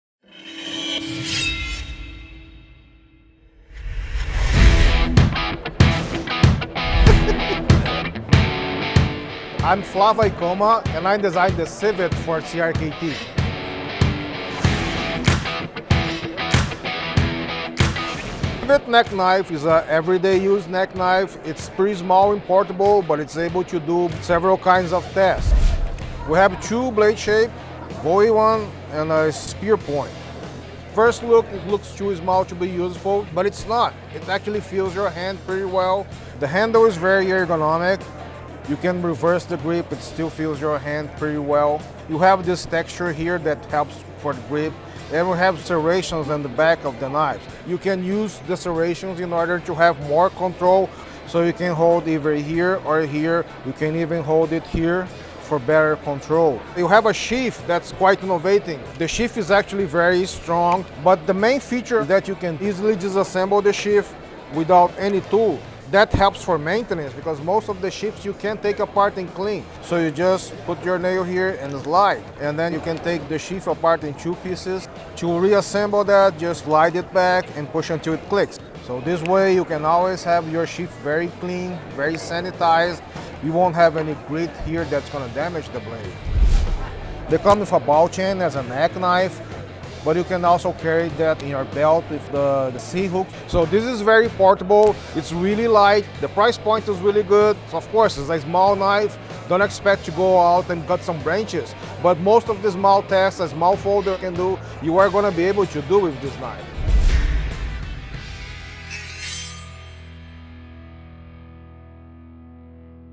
Civet Knife | Designer Vision From Flavio Ikoma
Tear through tasks with cat-like performance.

Tear through tasks with cat-like performance. The Civet™ is a fixed blade everyday carry neck knife. But like the predator it's named after, it won't sit still for long.

Flavio Ikoma of Presidente Prudente, Brazil, designed the Civet™. With several creative patents and innovations under his belt, he draws inspiration from things like sports cars, jet fighters, and stealthy animals. His latest blade features a new, innovative, Easy Clean, slide apart sheath is no exception.

The 2.4" Bowie-style blade goes back and forth between an all-purpose utility tool and stylish statement. On one hand it has the sophisticated lines that have made Ikoma famous. On the other hand, jimping on the spine of the blade provides you with enhanced cutting control. Add to that an ergonomically designed handle and the Civet™ is ready to be your sharp companion wherever you may venture.

What makes this blade truly revolutionary though is a sheath that can be taken apart allowing for easy and effective maintenance. Debris is gone. Which means rust and damage is easier to control. It's all so you can keep going through any job that you put your mind, or this blade, to.

If you want a blade that's compact, portable, and packs great performance, the Civet™ is ready to pounce on the task at hand.